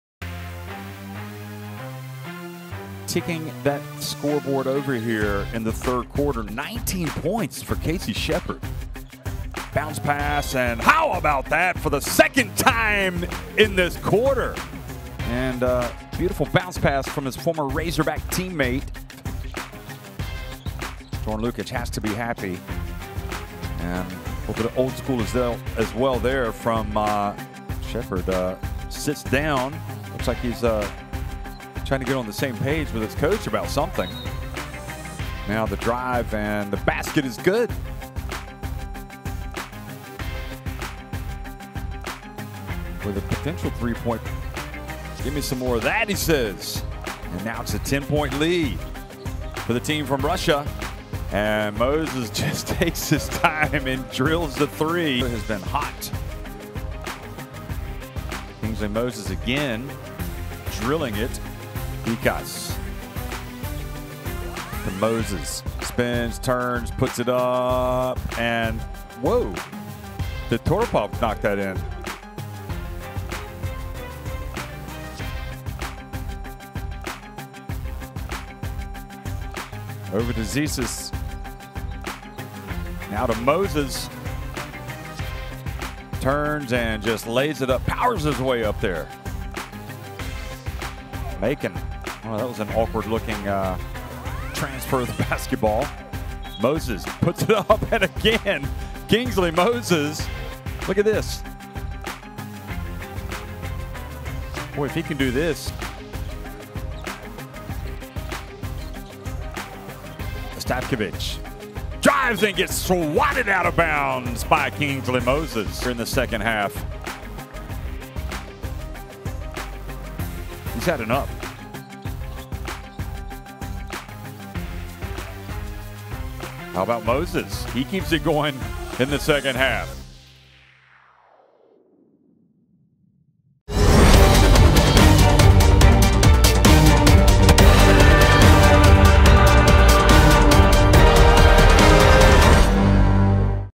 Moses Kingsley rocks the rim | Basketball Champions League 2020/21
Moses Kingsley leads AEK to a close victory over Nizhny Novgorod on Gameday 6 in the Round of 16 of the FIBA Basketball Champions League 2020/21 with 19 points and 6 rebounds. Enjoy his best plays!